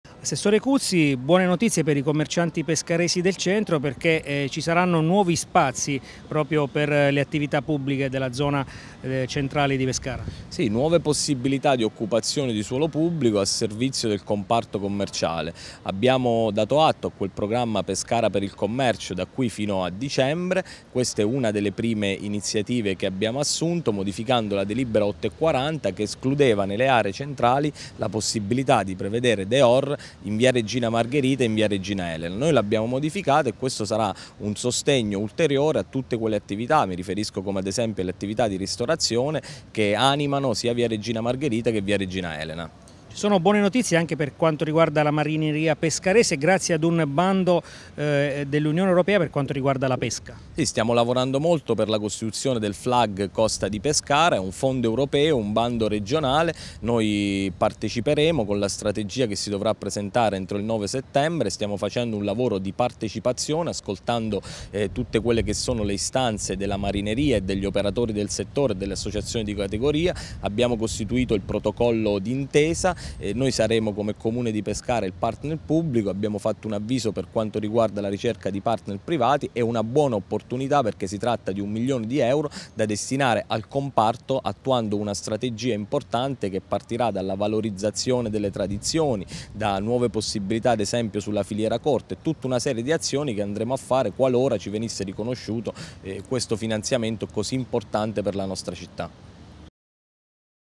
Dehors e Bando della Pesca per commercianti e marineria pescarese
Nuovi spazi in centro e occupazione di suolo pubblico per le attività dei commercianti di Pescara con Dehors. L'annuncio è arrivato dall'assessore Giacomo Cuzzi che ha illustrato anche le nuove opportunità per la marineria con il nuovo Bando della UE